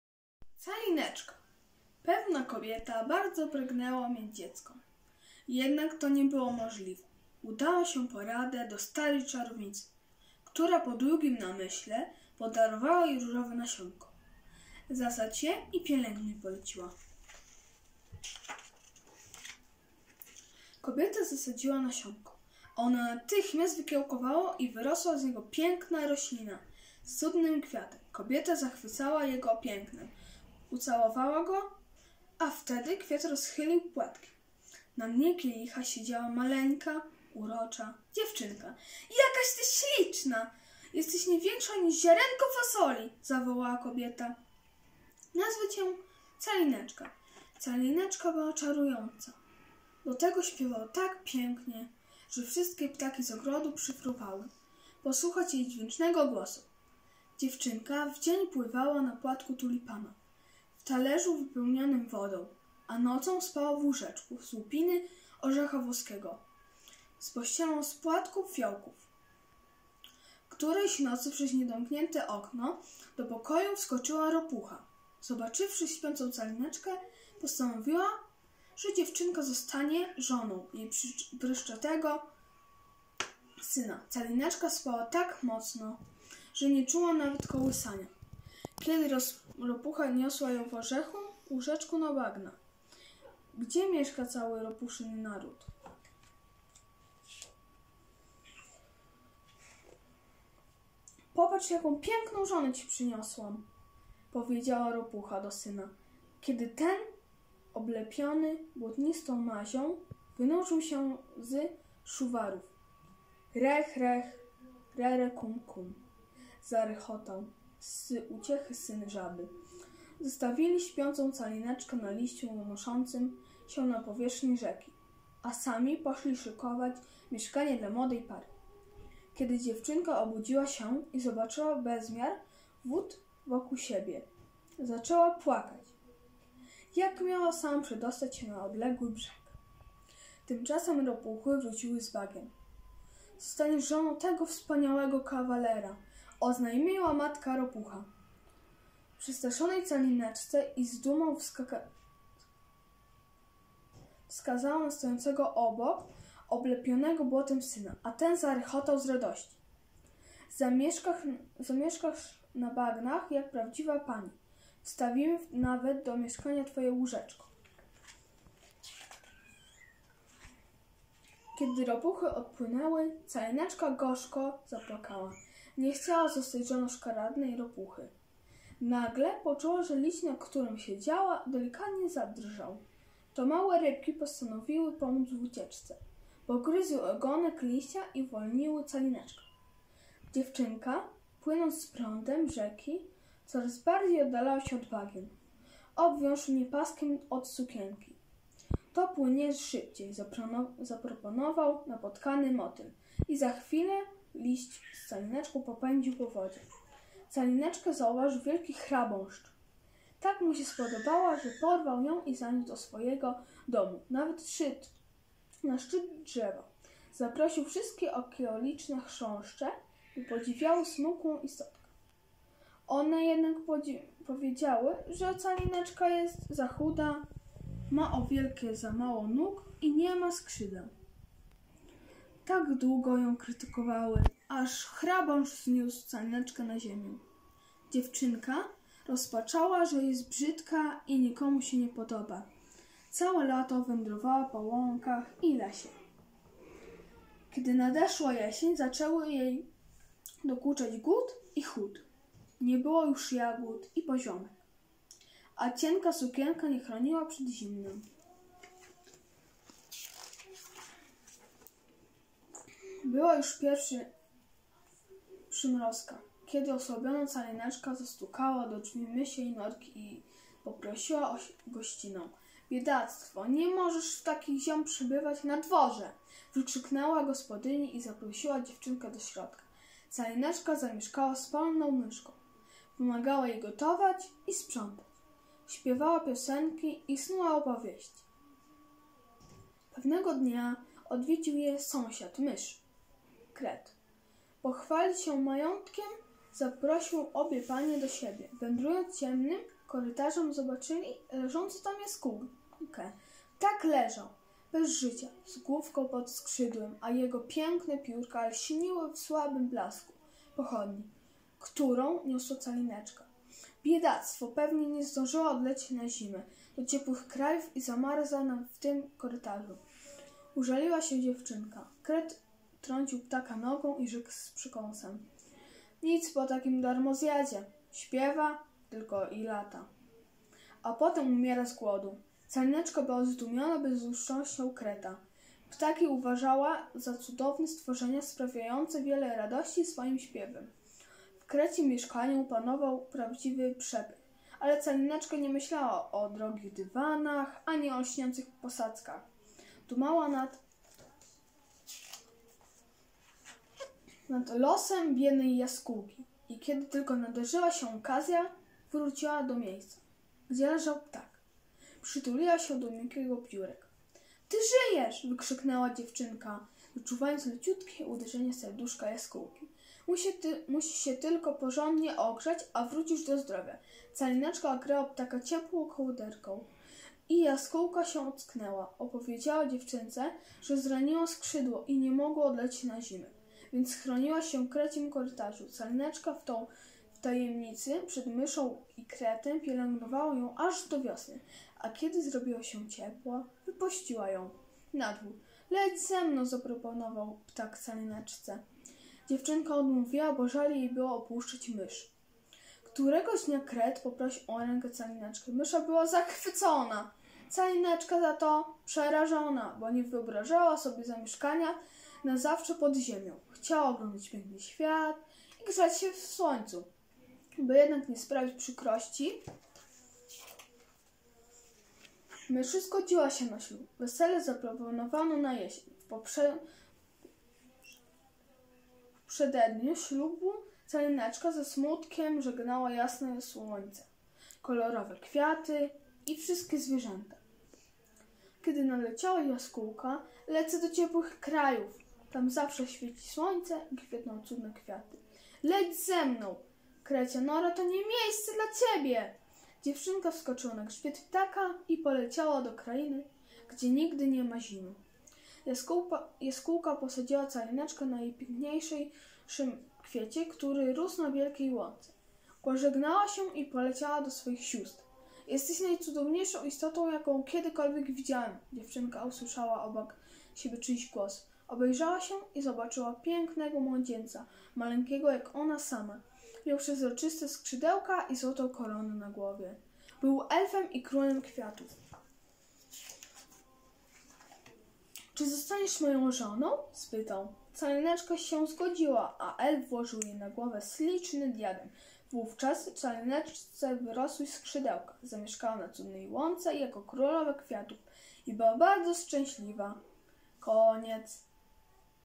Paweł, uczeń kl. IV z SP w Drozdowie czyta fragmenty baśni "Calineczka" H. Ch. Andersena
W ramach Dni Literatury Dziecięcej do akcji czytelniczej ph. "Baśnie, bajki i ballady z dziadkowej szuflady" zachęcamy wszystkich miłośników książek.

Fragmenty baśni pt. "Calineczka" H. Ch. Andersena czyta Paweł, uczeń klasy IV ze Szkoły Podstawowej w Drozdowie.